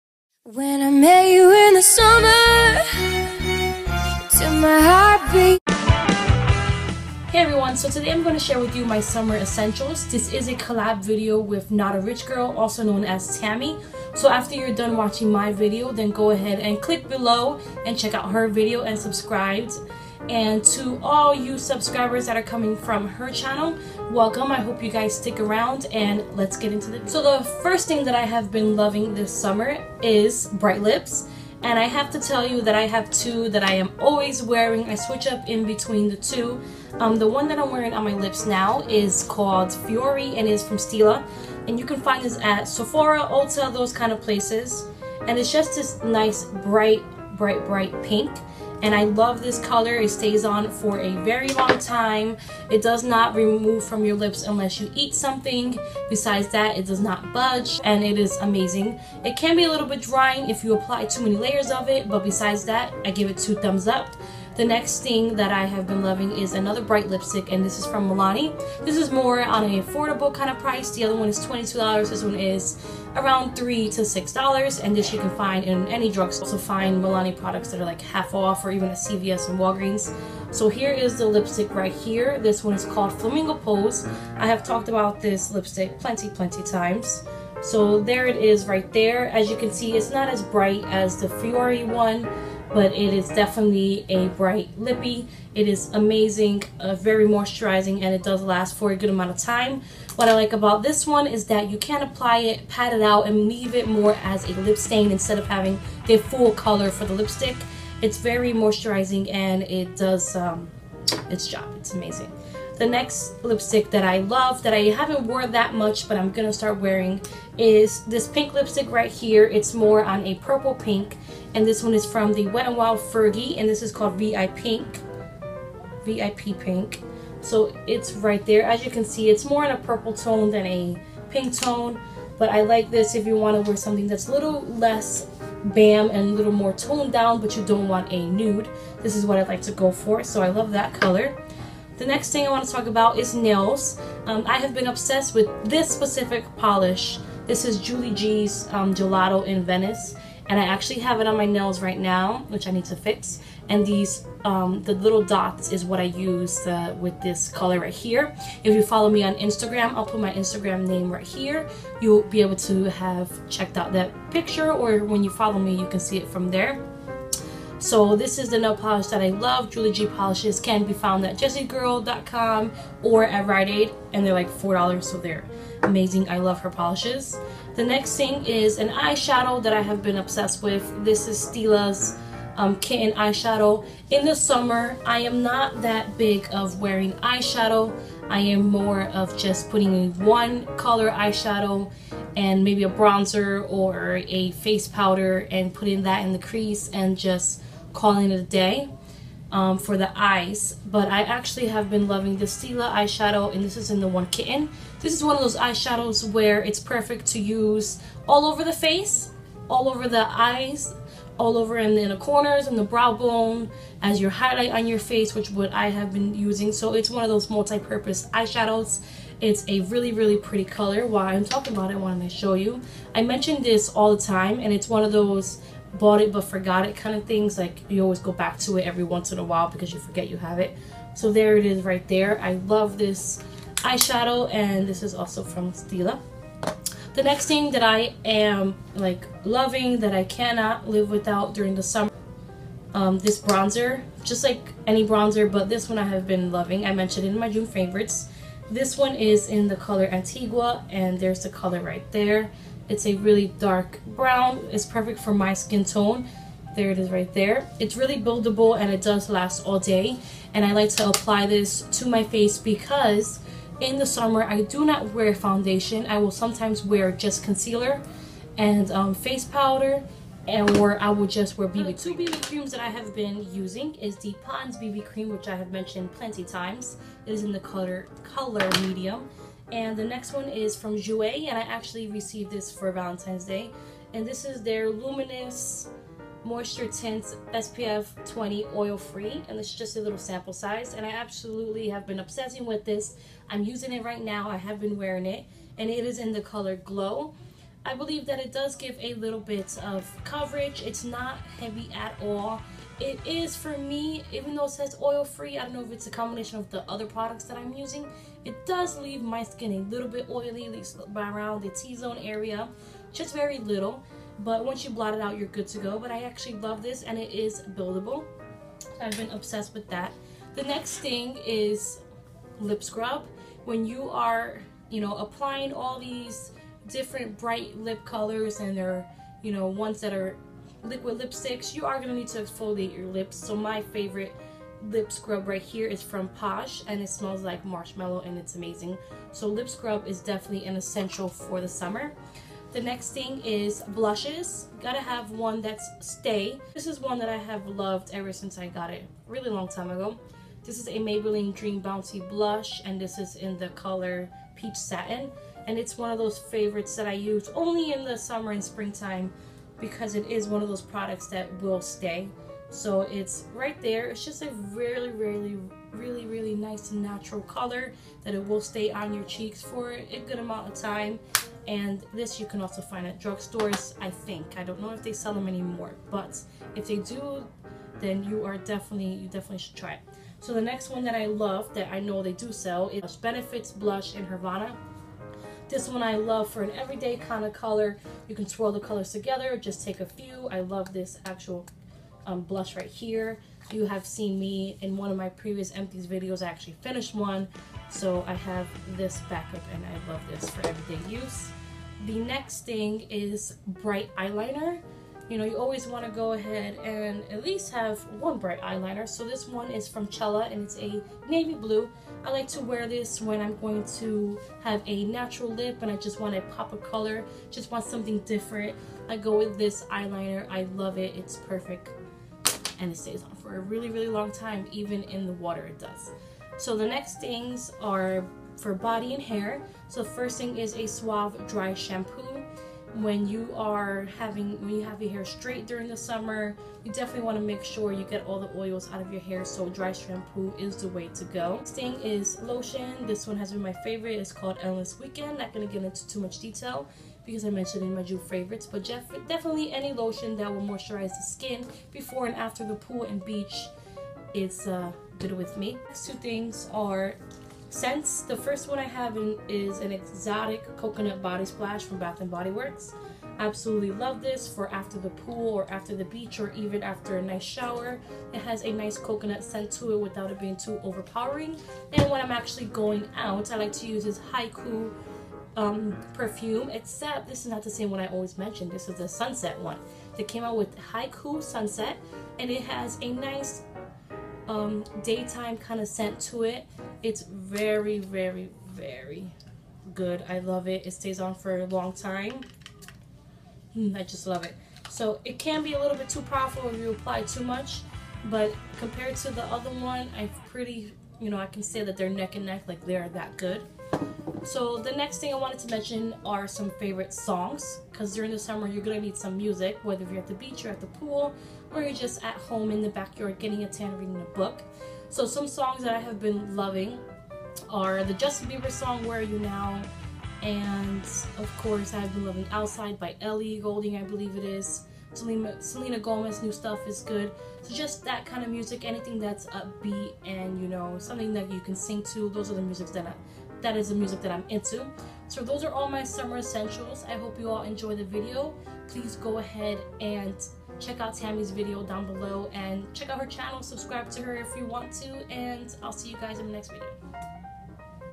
SUMMER ESSENTIAL'S COLLAB W/ NOTARICHGIRL!!
Hey everybody!! Today I have a collab with notarichgirl aka tammy! We've actually met before and she's such a nice person.im going to share with you all my summer essentials and then head over to her channel to check out hers and subscribe!